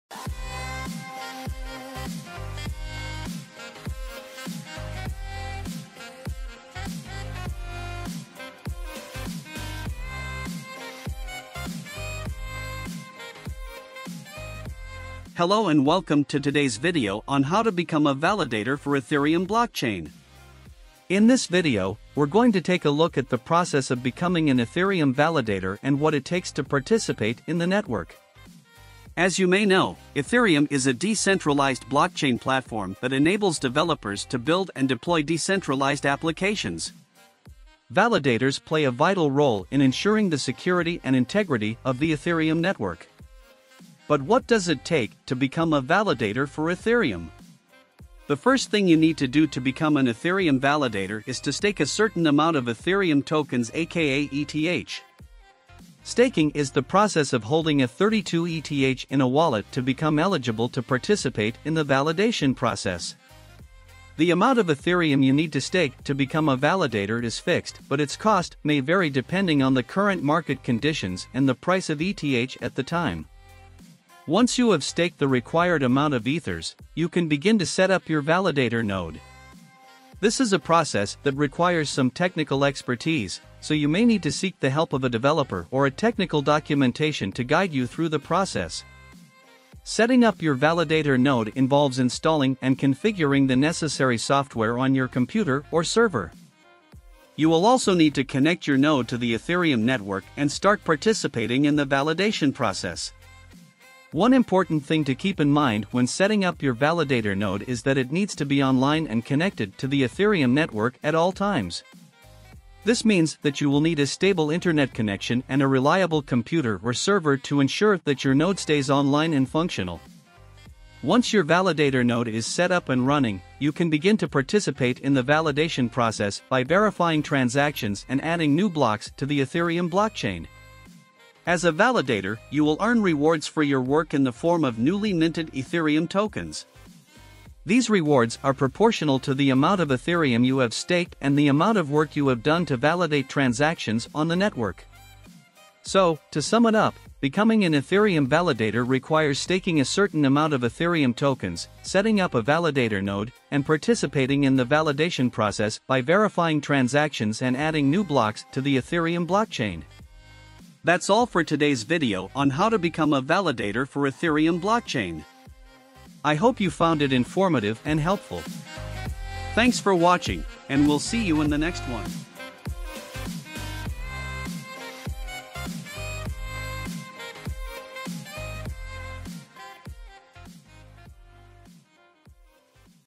How to Become Validator for Ethereum Blockchain?
Contents
0:14 - Intro
0:53 - Staking
1:23 - Setup Node
2:06 - Roles & Responsibilities
2:29 - Summary